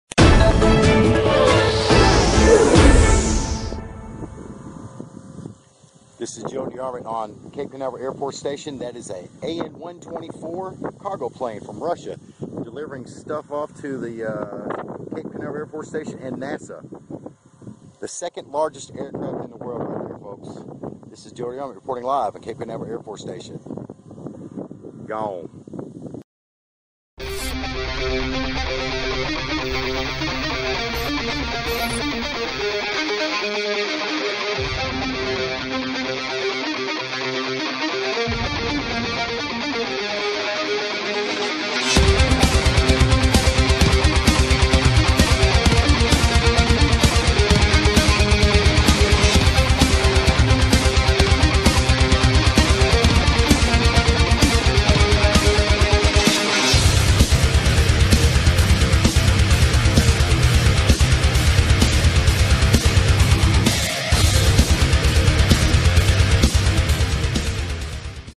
2nd largest airplane in the world
An124 landed at Cape Canaveral airforce  station few years back.take off footage added in tbis 2019 remixed short video